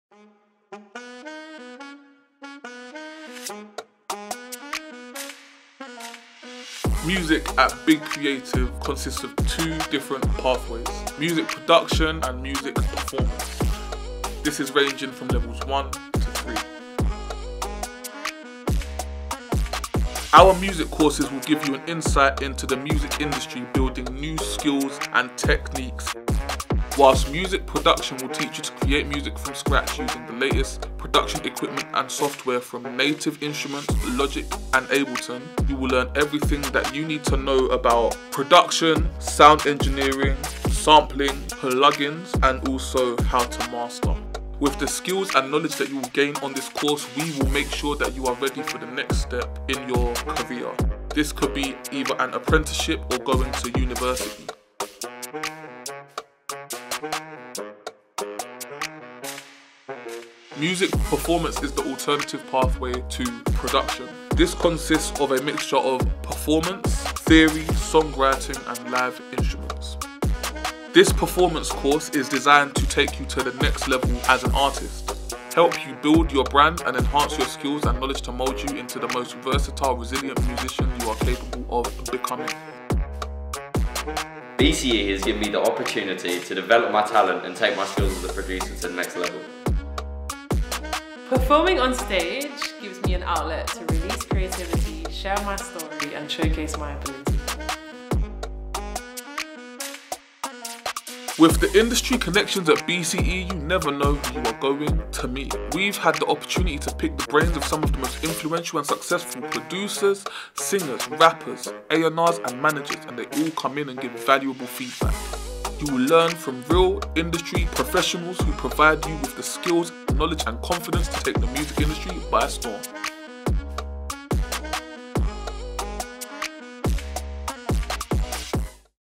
Music (Production & Performance) at Big Creative
Find out more about the vocational music courses on offer at Big Creative Education, ranging from levels 1 to 3.